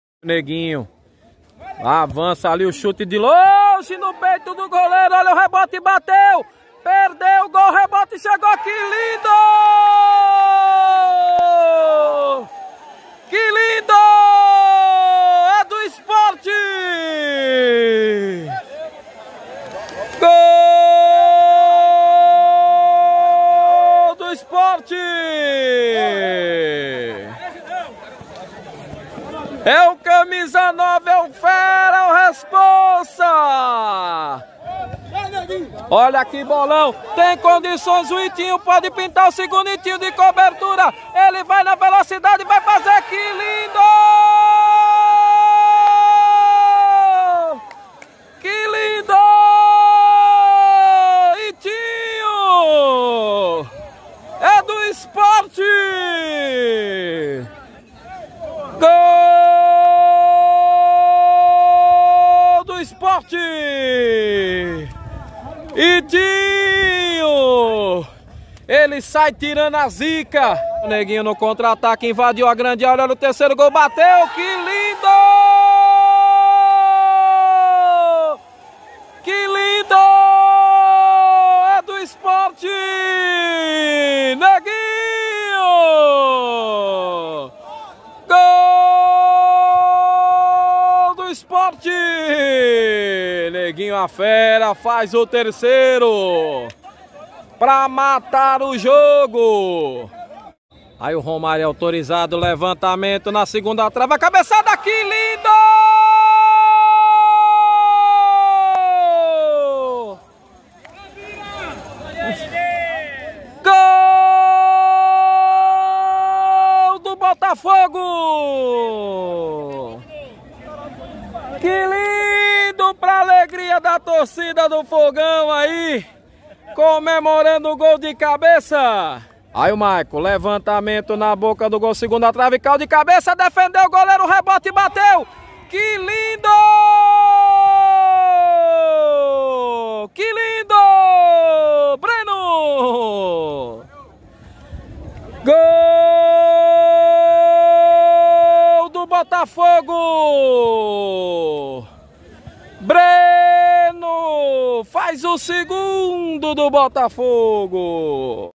OS GOLS DE SPORT 3 X 0 INTERNACIONAL E BOTAFOGO 2 X 0 VARGINHA PELO CAMPEONATO DE ITAPISSUMA 2022
gols de jogos decisivos no campeonato aberto de Itapissuma 2022